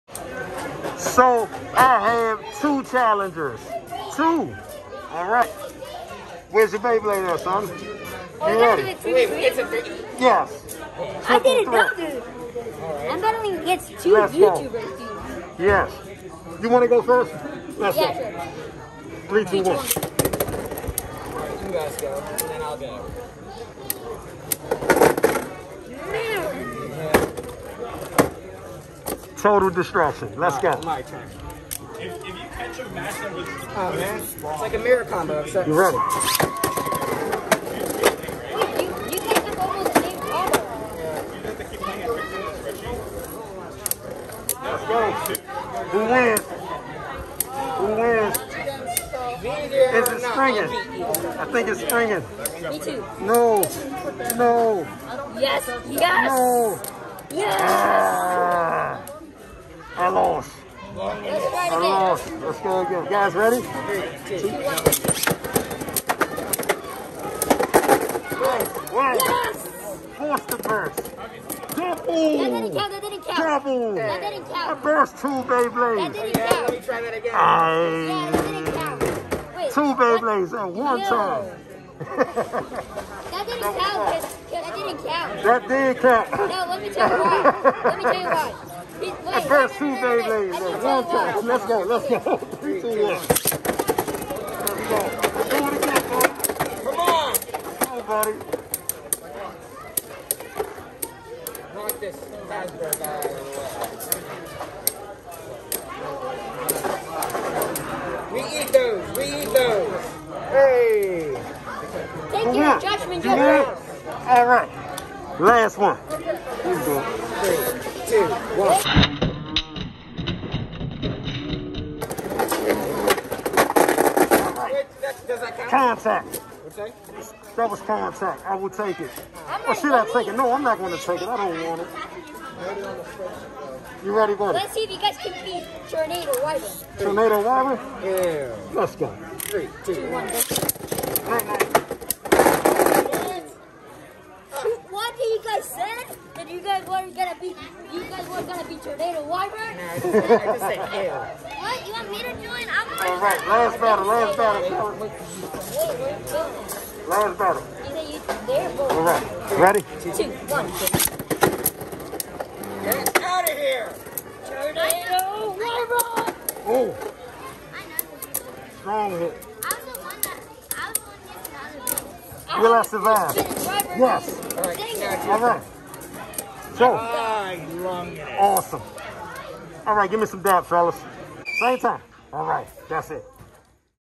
Beyblade Tournament Behind The Scene Battles August 22, 2021
These Tournaments Are Pure Fun!!
Shoutout to the young men whom battled Us
You guy’s are awesome!!❤️

Enjoy Your Day And Don’t Let Nobody Steal Your Greatest!!!

More unboxings and experiments are coming.
Thanks Guy’s.
LEAVE A 👍 If like our vids
Out!!